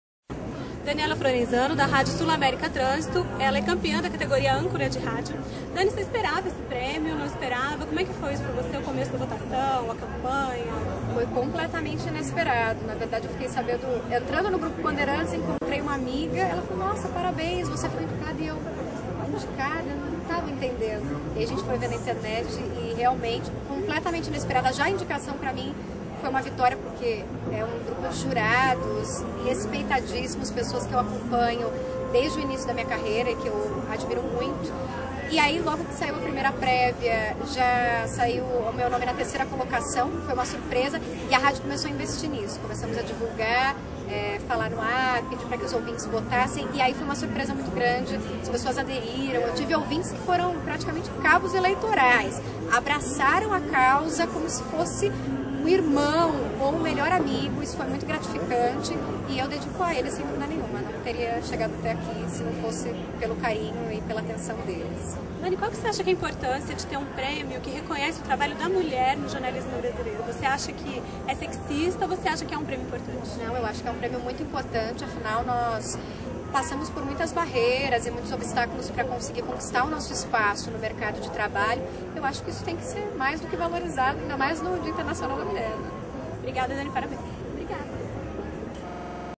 Daniela Florenzano, da Sulamérica, em entrevista a Thaís Naldoni, no 6º "Mulher Imprensa
Daniela Florenzano, da Rádio Sulamérica Trânsito, concede entrevista a Thaís Naldoni, na sexta edição do "Troféu Mulher Imprensa